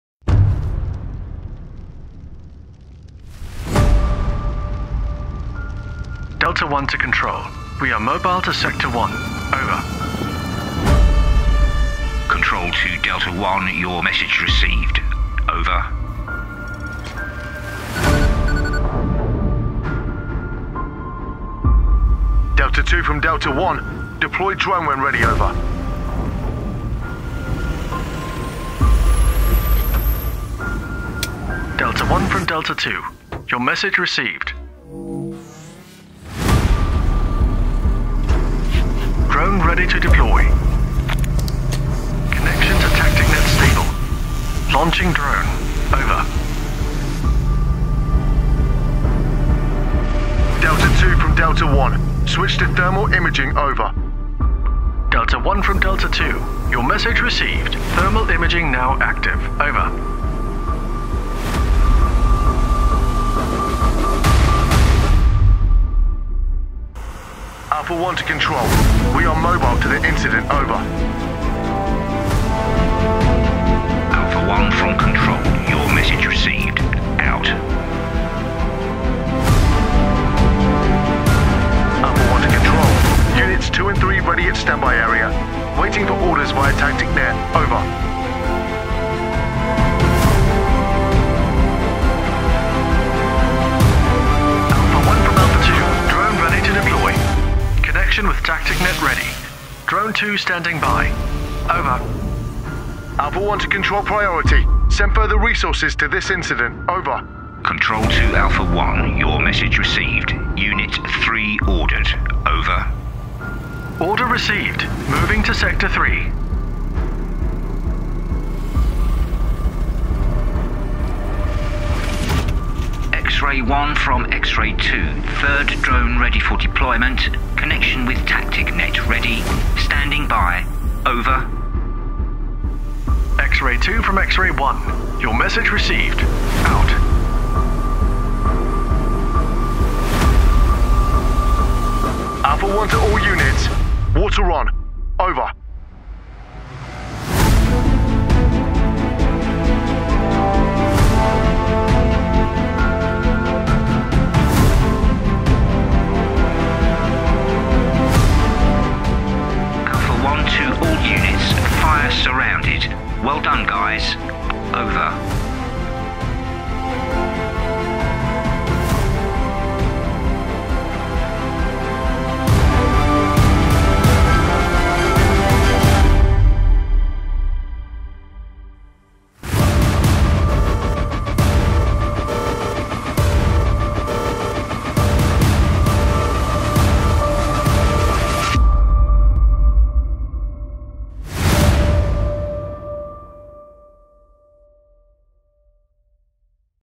Magirus TacticNet (Italiano)
Operazioni antincendio più efficaci e decisioni più rapide grazie alla visualizzazione e alle informazioni in tempo reale: con TacticNet, Magirus presenta una panoramica senza precedenti dell'intera area operativa. Visualizzazione, trasmissione dei dati e comunicazione in tempo reale in operazioni complesse, indipendenti dal luogo e ampliabili a piacere: tutto questo diventa realtà con la nuova rete operativa TacticNet di Magirus, basata sul WiFi. Per mezzo di mappe, immagini e materiale video HD, TacticNet fornisce per la prima volta ai comandanti, così come ai Vigili del Fuoco collegati e alle task force, una panoramica completa dal vivo, stabilisce possibilità completamente nuove per il comando tattico, il controllo e la comunicazione e consente decisioni di missione rapide e mirate. Grazie all'integrazione di un'ampia gamma di fonti di informazione, il comandante dell'incidente e i responsabili delle decisioni a tutti i livelli hanno accesso a tutte le informazioni rilevanti in modo rapido, completo e anche in circostanze difficili come una visibilità limitata o condizioni del terreno difficili. È la base per il processo decisionale in situazioni operative complesse come incendi di vegetazione, controllo delle catastrofi, operazioni di salvataggio e industriali o altre missioni impegnative. Entrate a far parte della prossima generazione, la Next Generation Firefighting di Magirus, e scoprite di più su TacticNet su